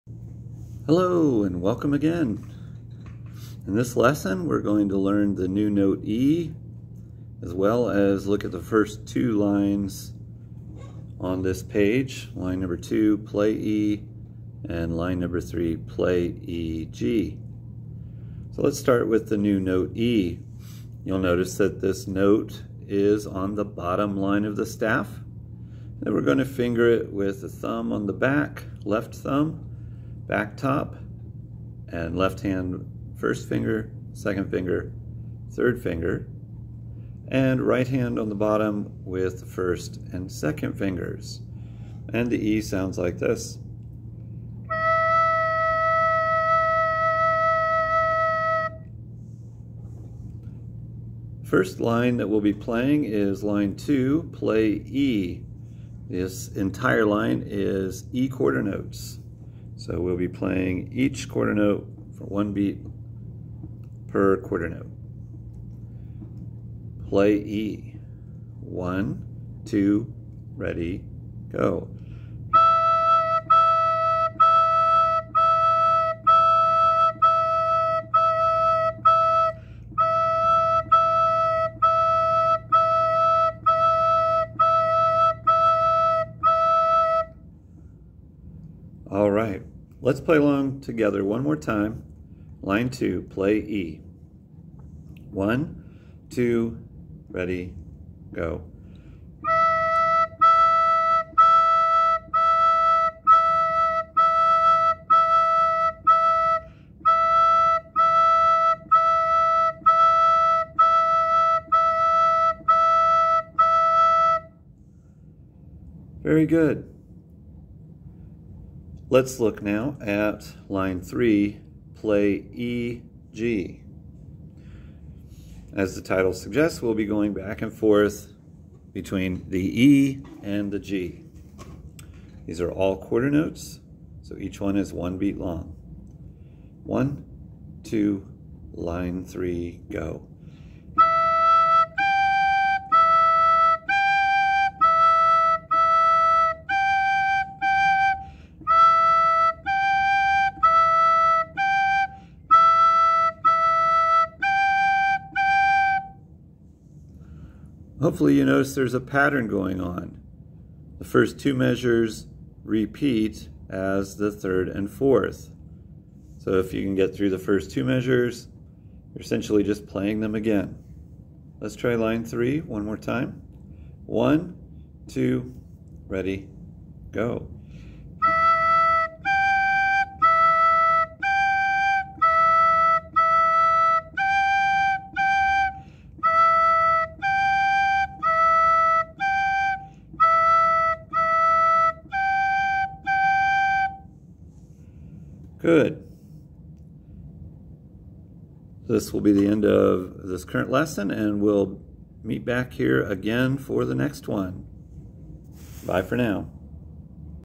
Recorder Lesson 8: New Note E, Play E, and Play E, G
Learn the new note E.